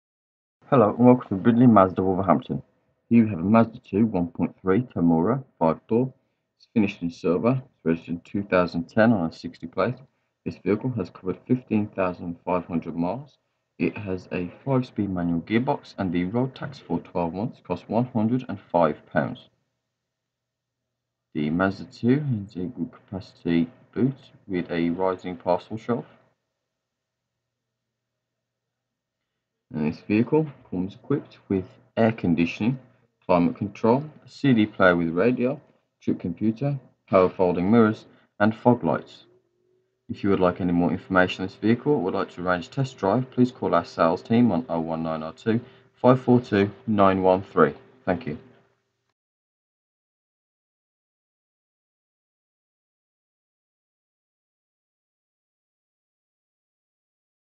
Mazda 2 1.3 Tamura 3 Door DU60 YOL
This car is finished in Silver, has covered 15513 miles and comes with a full service history.

This car comes with Electric Windows, Electric Mirrors, Air Conditioning, CD Player, Trip Computer, Central Locking, AUX Input, Power Mirrors, Fog Lights and Alloys.